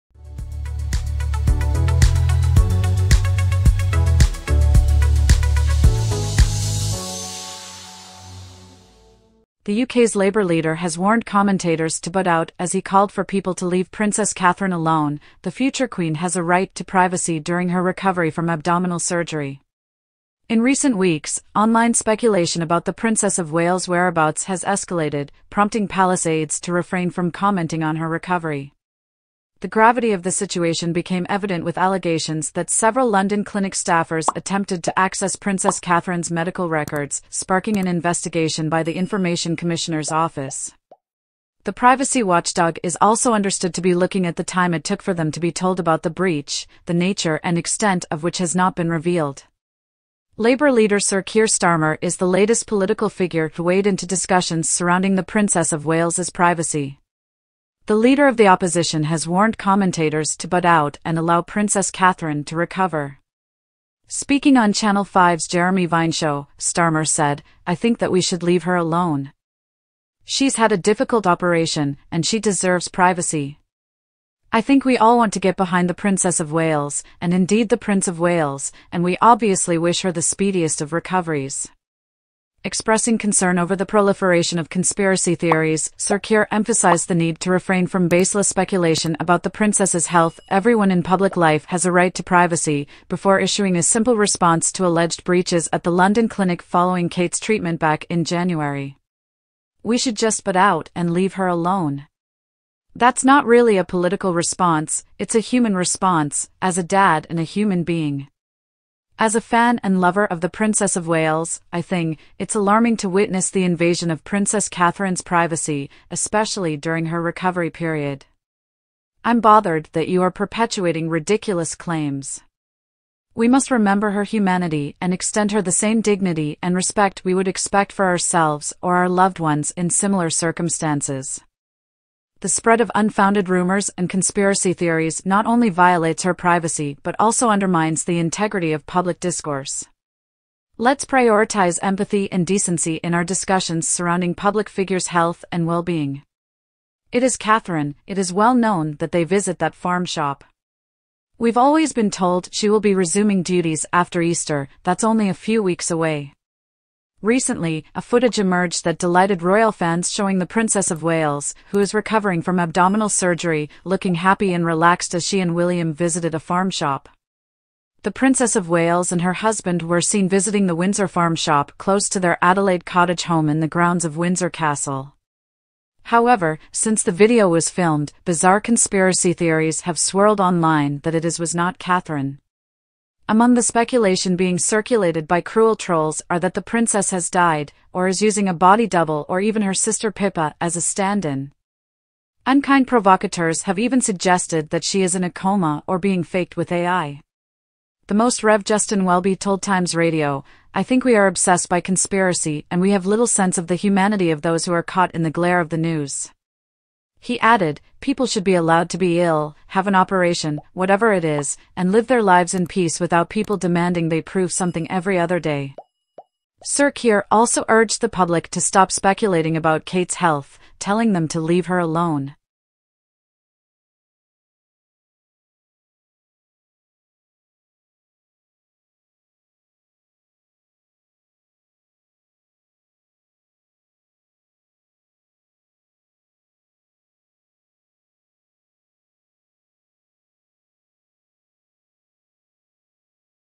The CRUEL TRUTH of Princess Catherine's PRIVATE PAIN After Surgery Made Us Speak Up
The CRUEL TRUTH of Princess Catherine's PRIVATE PAIN After Surgery Made Us Speak Up @HRHcatherineprincessofwales
👉You will love this video: Inside The Palace's BIG PLAN for Princess Catherine's Return to Public Life After Surgery